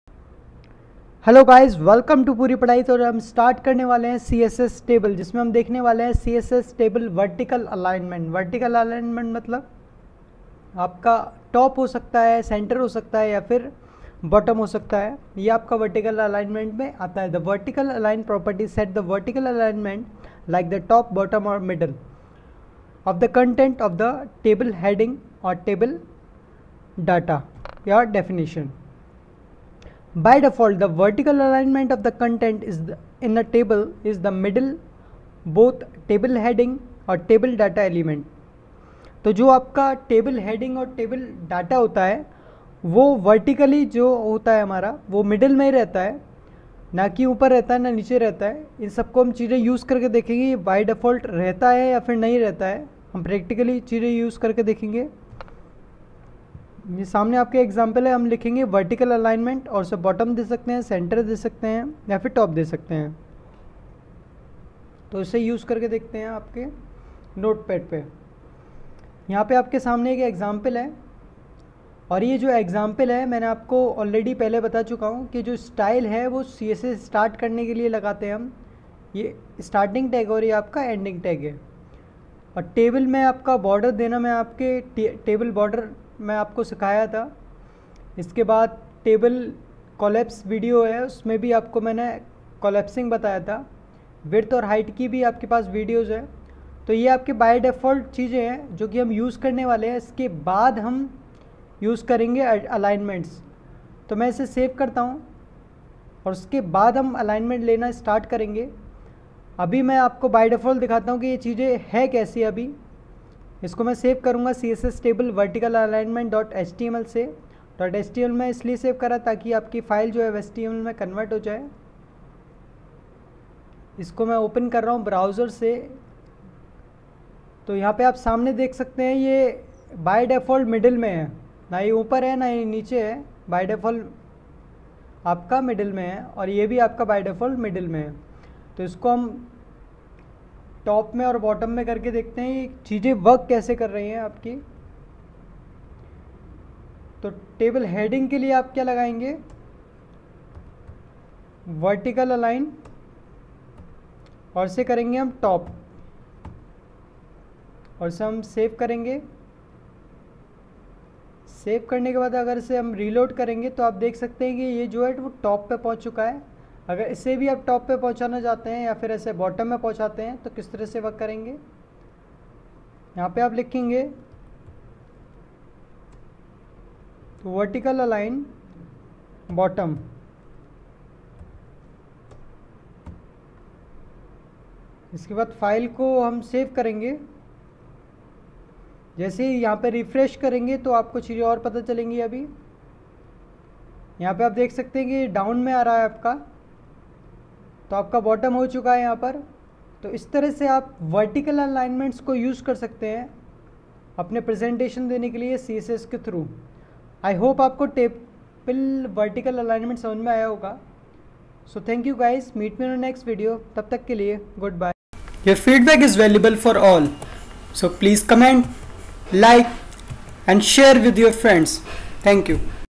CSS Table Vertical Alignment-Hindi
What is the use of CSS Table Vertical Alignment and how to use it to make website more beautiful.

You can download all the examples from 1st video of CSS Tutorial-CSS Overview